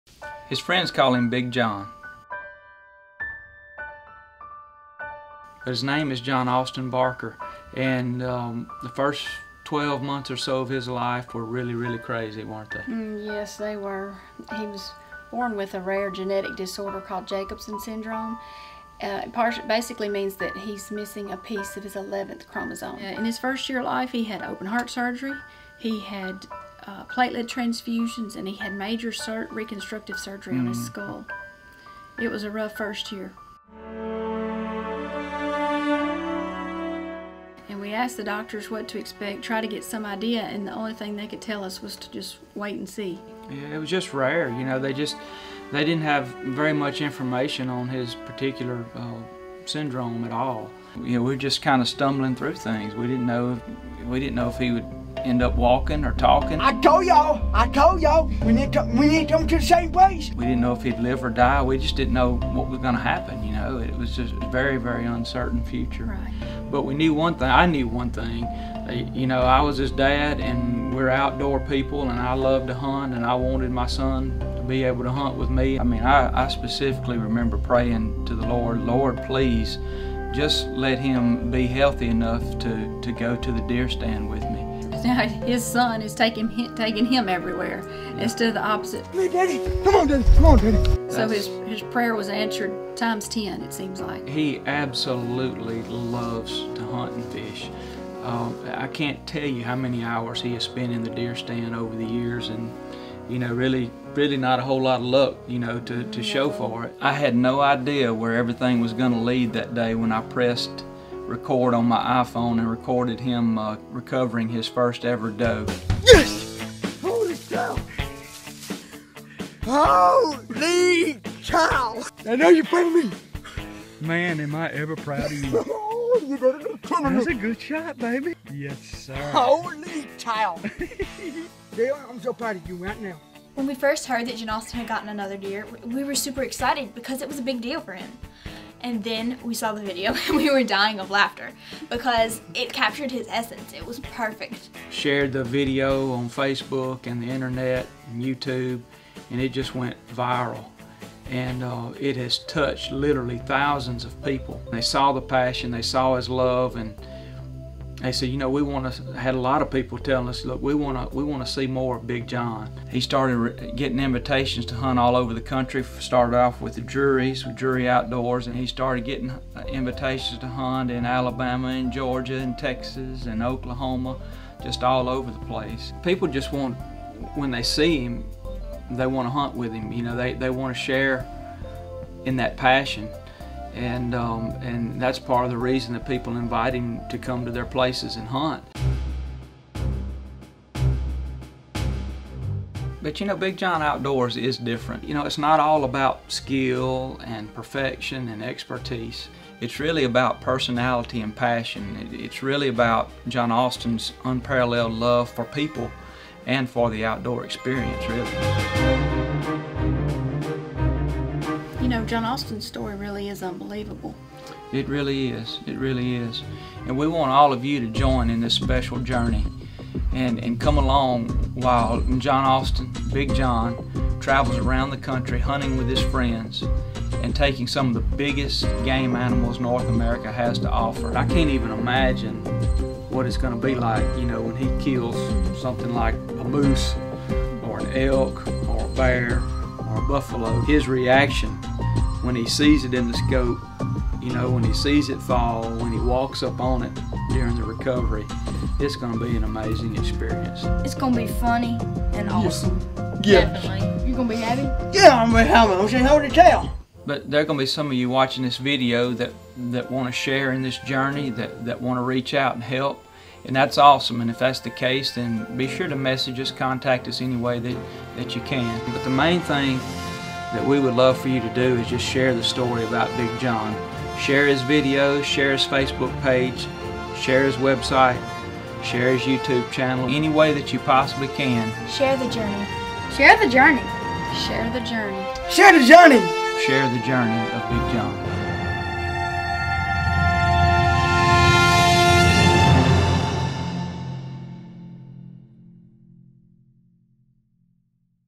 The Story of Big John...the most AMAZING underdog STORY in the Outdoor Industry!
The outdoor industry's most AMAZING underdog STORY. a tale of Triumph over Tribulation! Watch this short video to see what Big John and Big John Outdoors is all about!